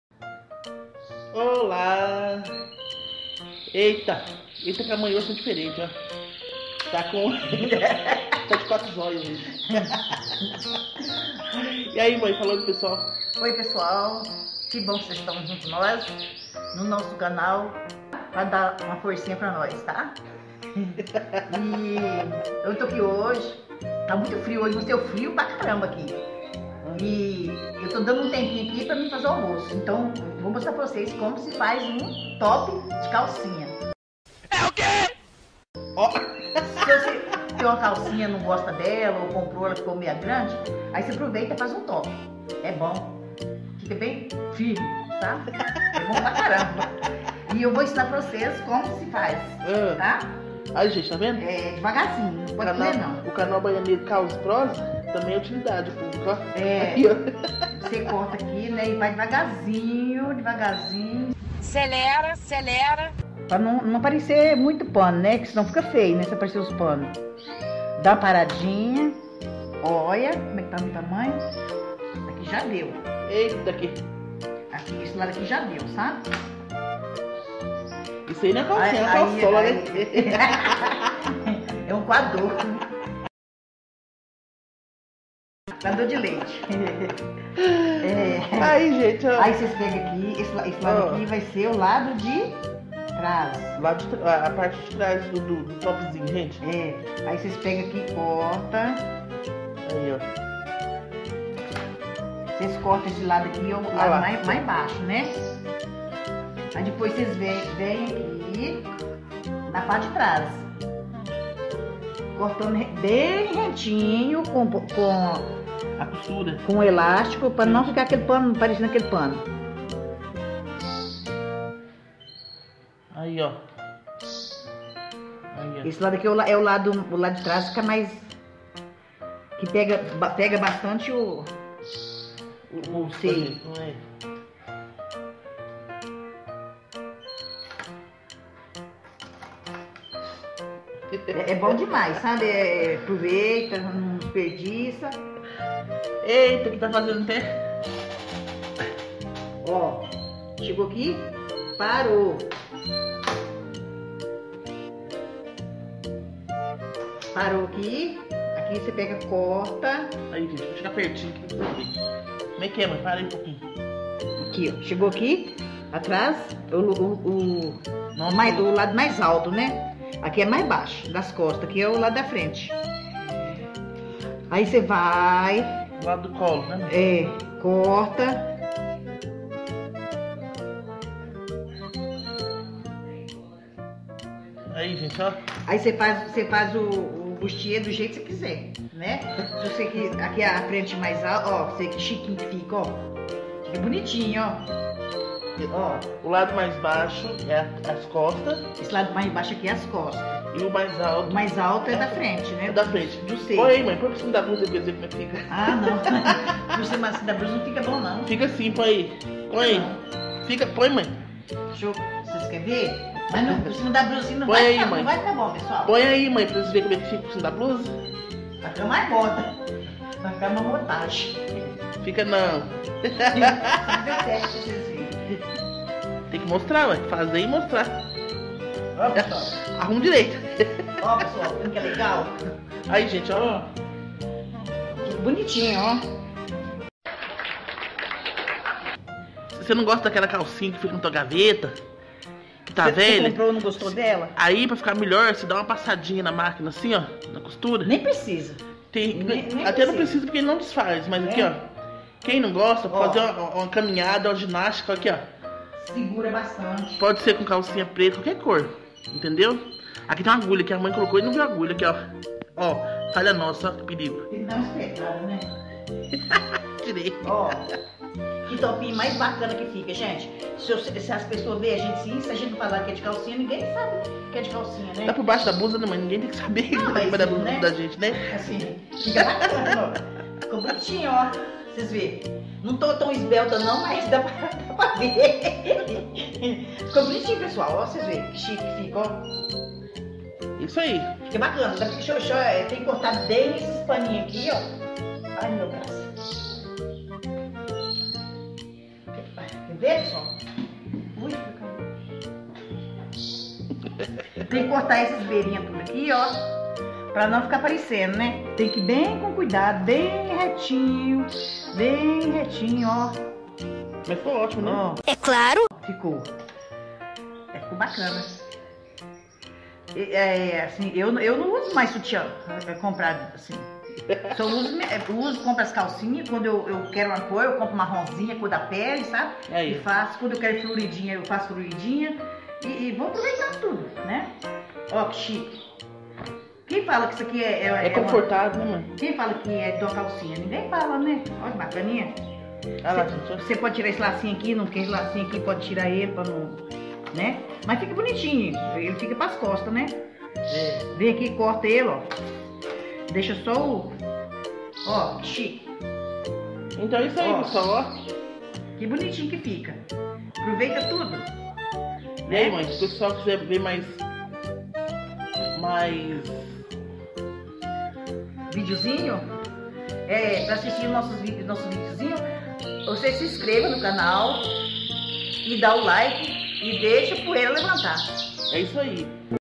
Iza transforma calcinha em um belo top, muito TOP kkkkkk
Você tem uma calcinha que não te agrada ou não serve mais? Que tal transformá-la em um belo TOP? Iza e Patricia mostram como você pode fazer isso com muita alegria e descontração kkkkkkkk.